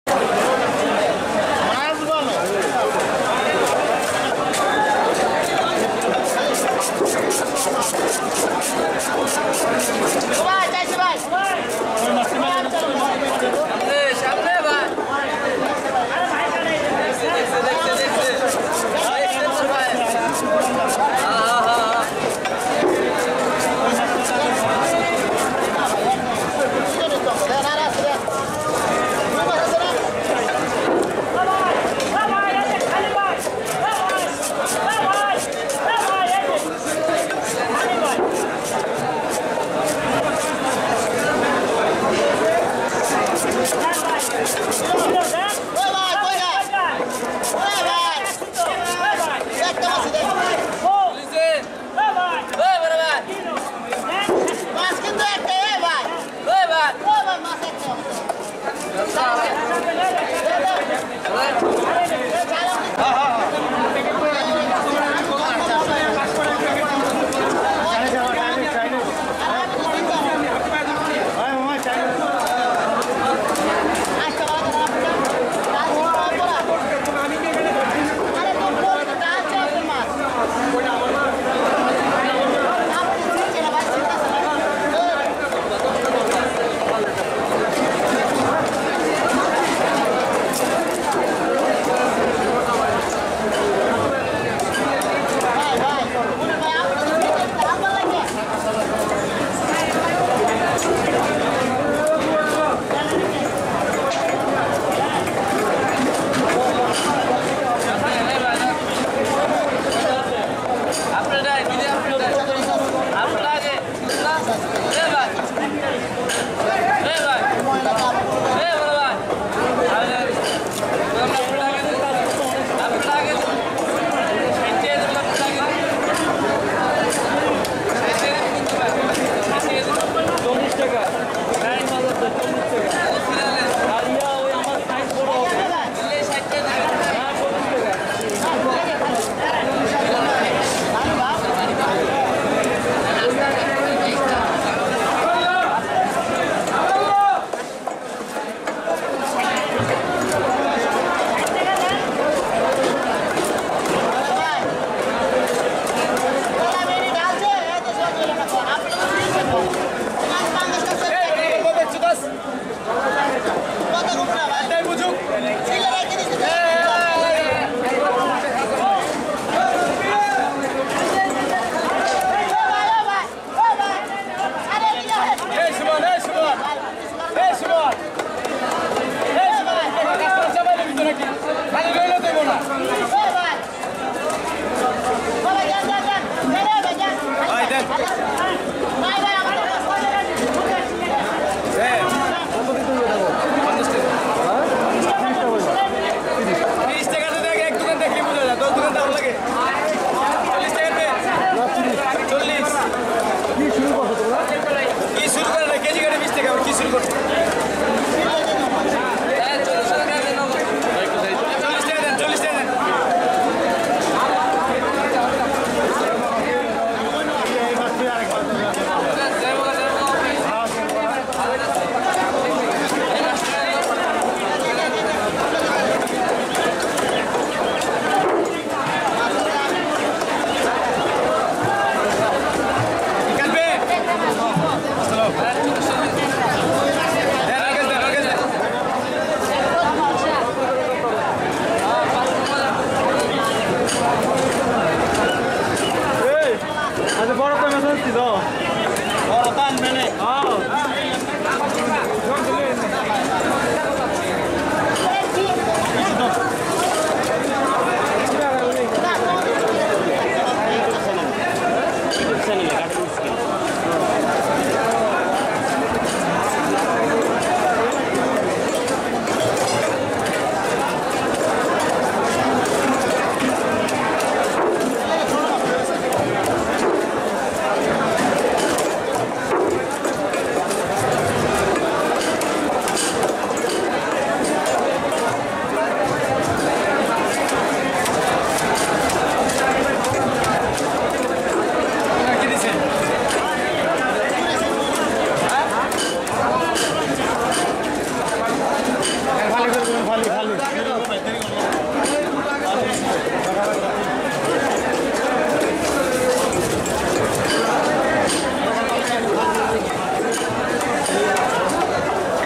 Big Silver Carp Fish Cutting In Bangladeshi Fish Market Fastest Fish Cutting Skills By Expert Fish
Big Silver Carp Fish Cutting In Bangladeshi Fish Market Fastest Fish Cutting Skills By Expert Fish Cutter